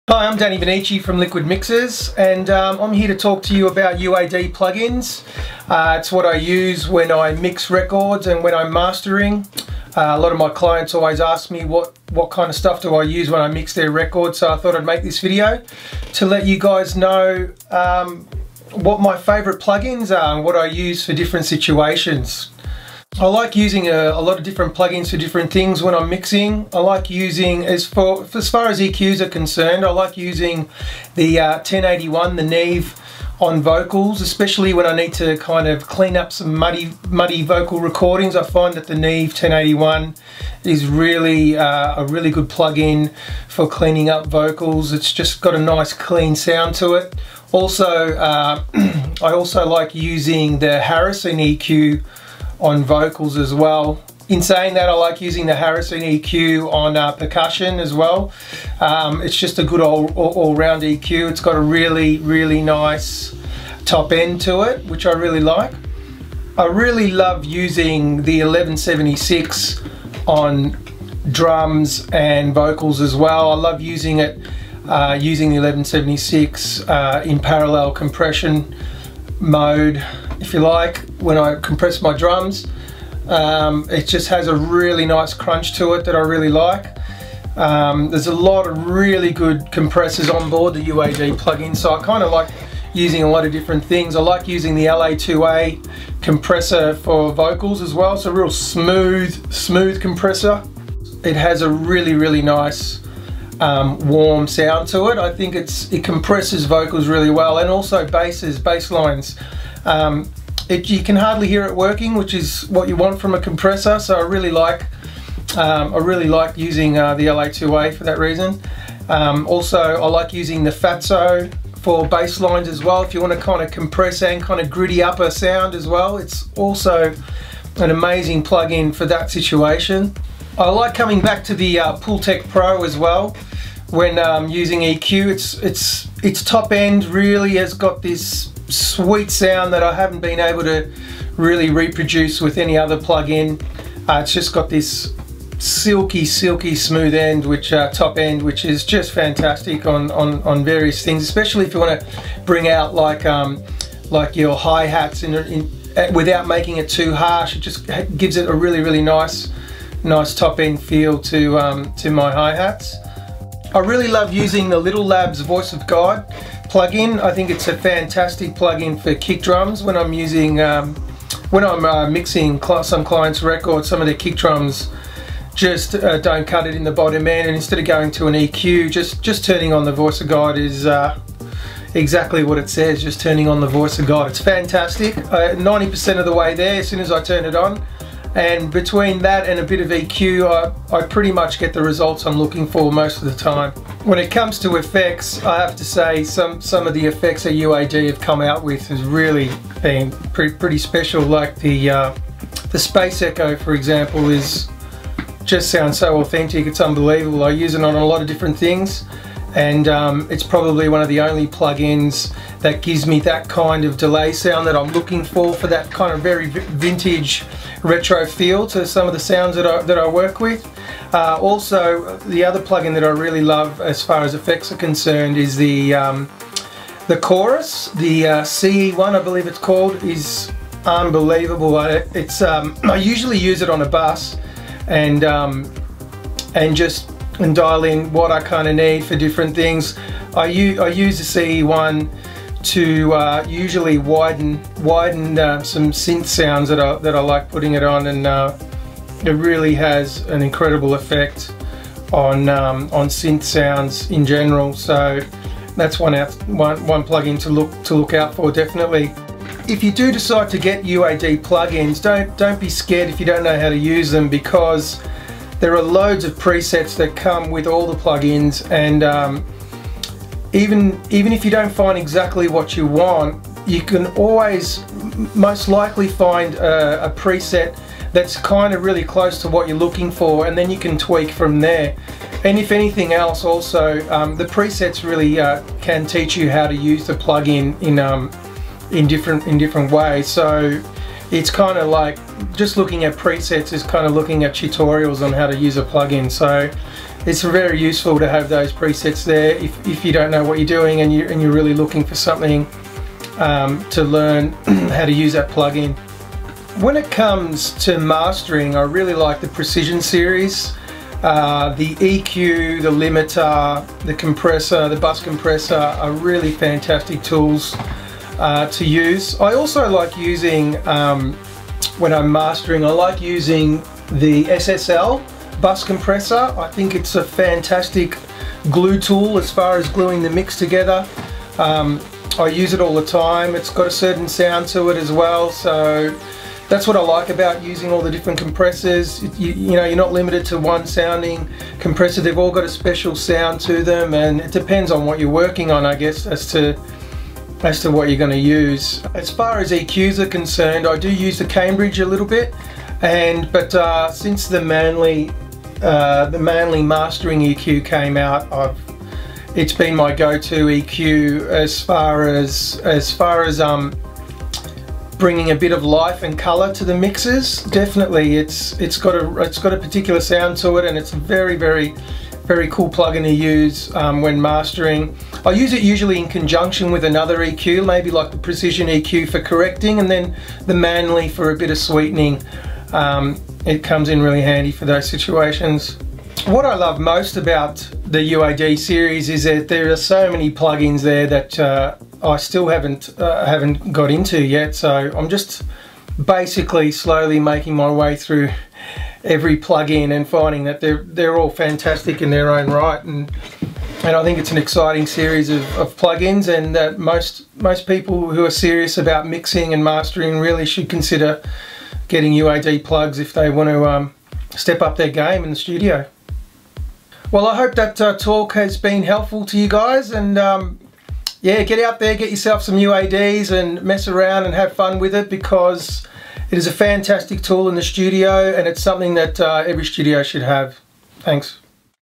Dannybonnici talks about UAD
interview about UAD plug ins